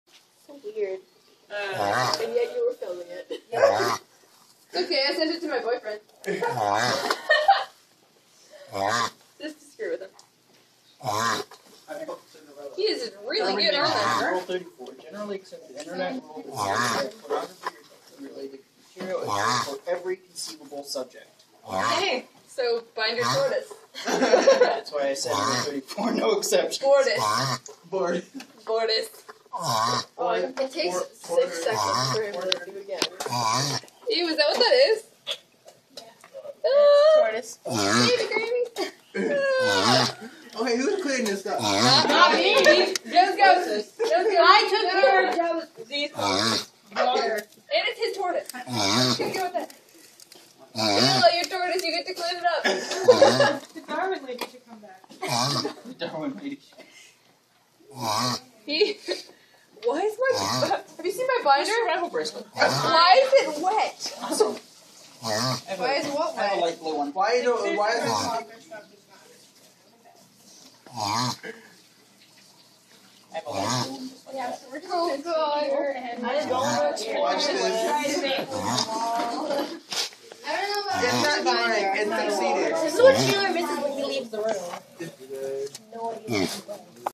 Sulcata Tortoise "Darwin" Trying To Mate With A Binder
Darwin, our Sulcata Tortoise has been behaving strangely, trying to crawl into a low shelf where some notebooks are kept.  We finally found out why when he began doing this!  Please help us care for the animals